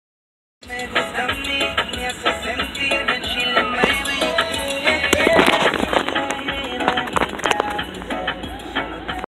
SAFD Ladder Responding
SAFD Ladder Responding Couldn't See The Number Sorry About The Music Tho It's Very Hard When Im In The Back Seat And My Sister Is Playing She Doesn't Like To Turn It Off.Taken On 6/22/17